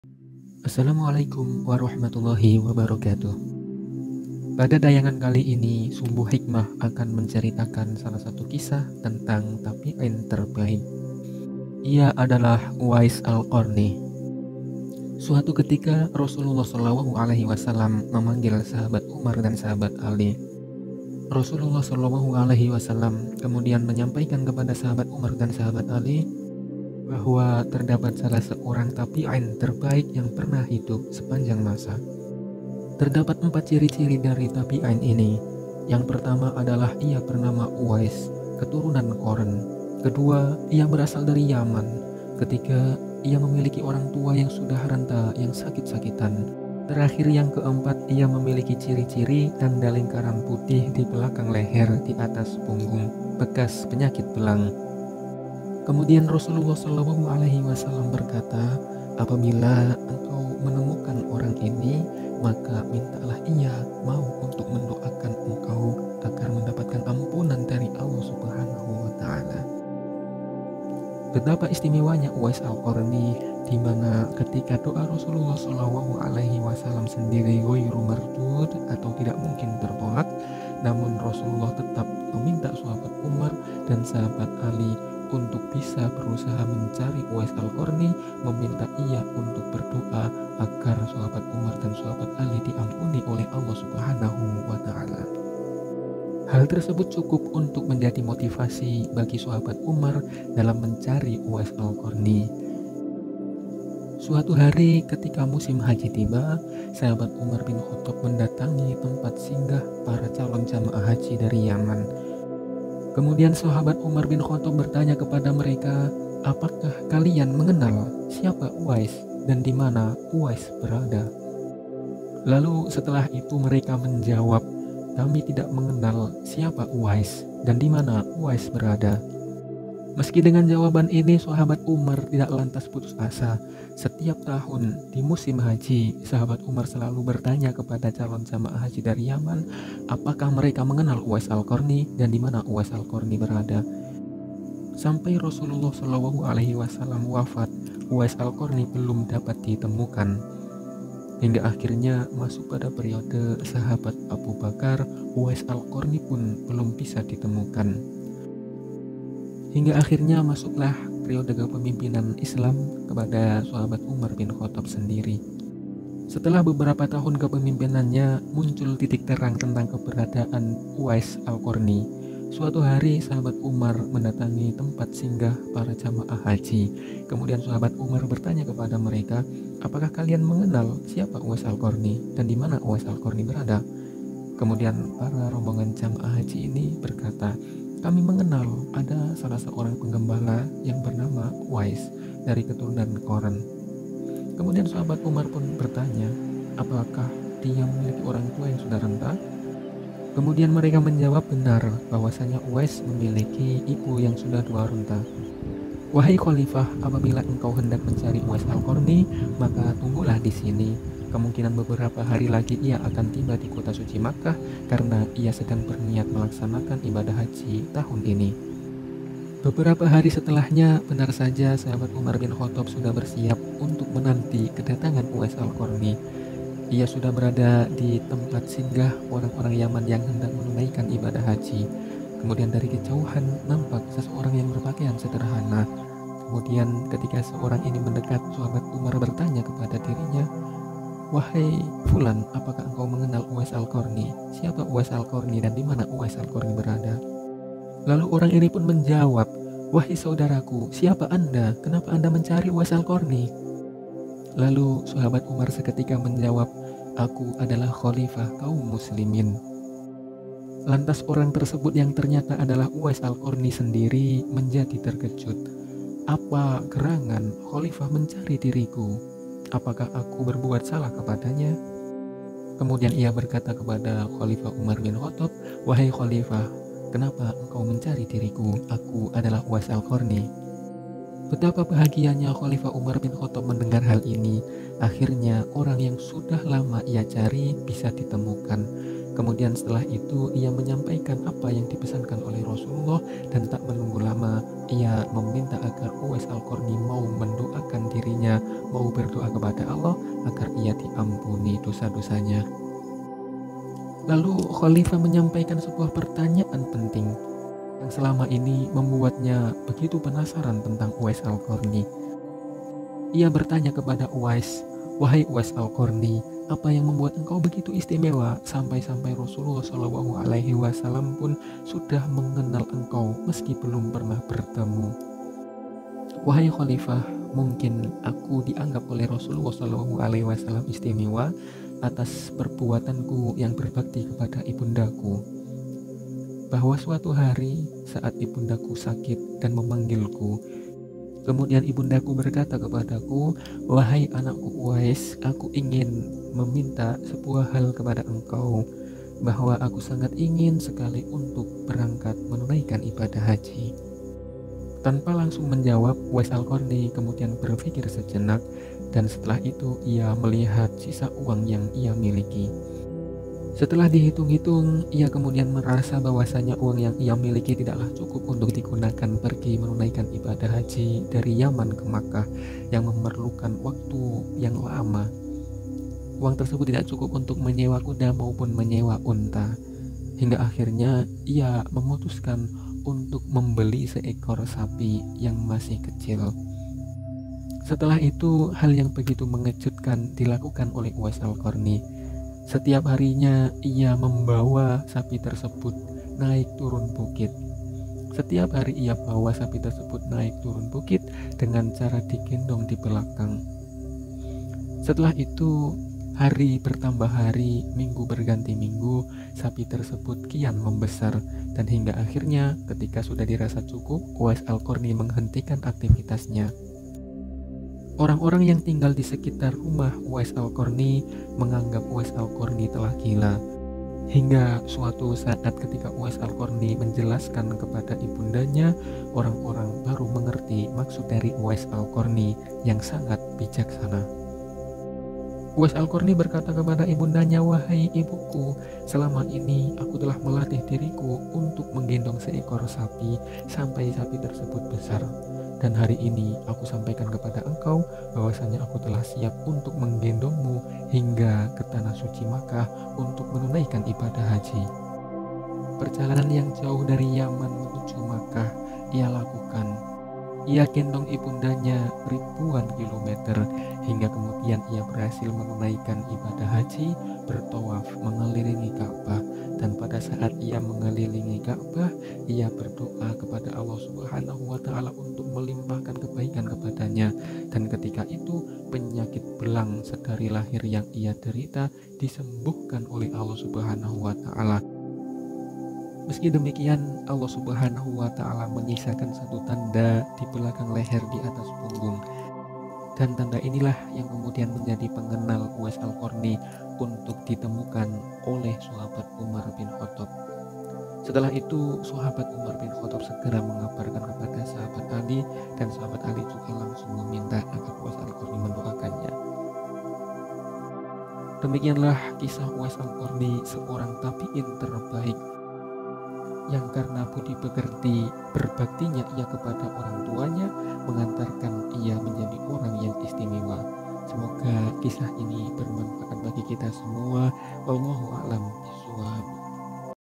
Kisah Uwais Al Qarni Tabiin Terbaik yang Menggendong Ibunya dari Yaman ke Makkah Tanah Suci
Uwais Al Qarni Al Yamani Tabiin Terbaik yang Menggendong Ibunya dari Yaman ke Tanah Suci

kisah para rasul, kisah para Nabi dan rasul,